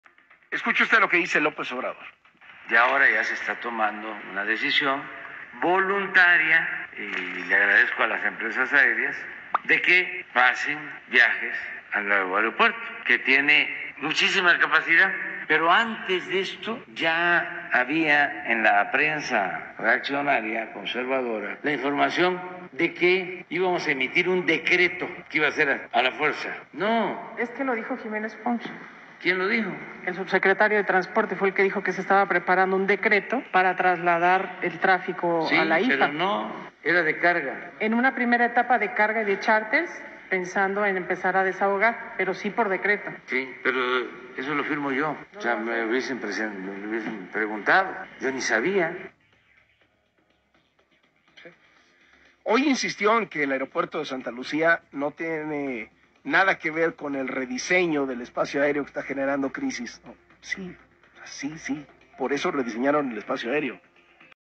YA SALIÓ EL PEINE! AMLO SE CONTRADICE SOLITO! YA ESTÁ MUY LOQUITO EL POBRE! SUS CHAIROS LLORAN!
En la 4T se contradicen entre ellos mismos, aquí en el canal de Morrillo Moreno te lo demostramos